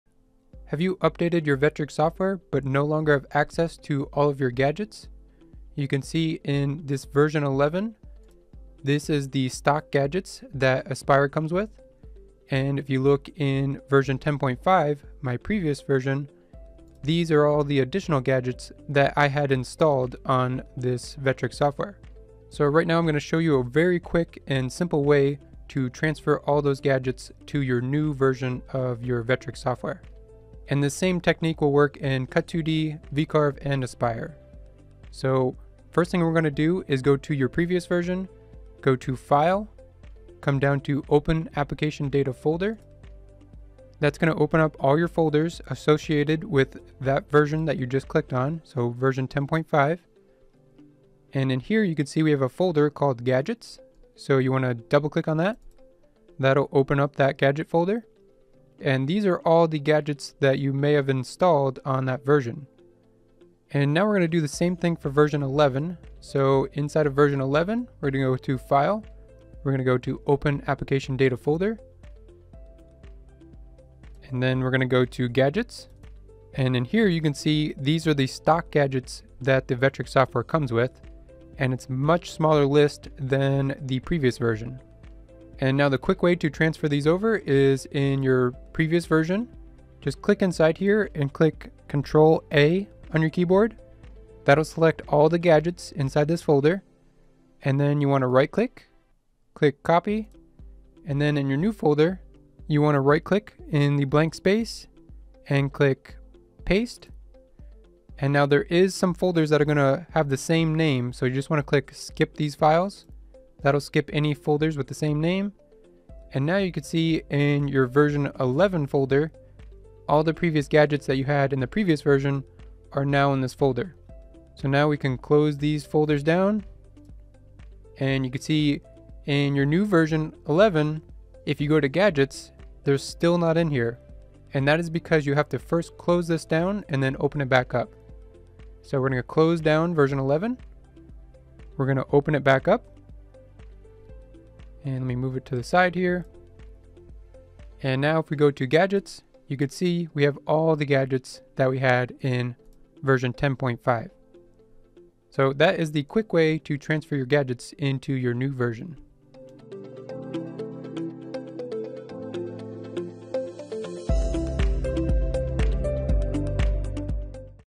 How to Transfer Gadgets to New Version of Vectric - Vectric VCarve, Aspire, & Cut2D Quick Tip
Quick tip on how to transfer your gadgets from your old version to your new version of Vectric software. This works for Cut2D Pro, VCarve Pro, and Aspire.

**Please note: Personal Gadgets are stored in the folder C:\Users\Public\Documents\Vectric Files\Gadgets\Aspire V11.0 (with a different version number on the end for each version). Use this folder location if you cannot find the gadgets that you have added on.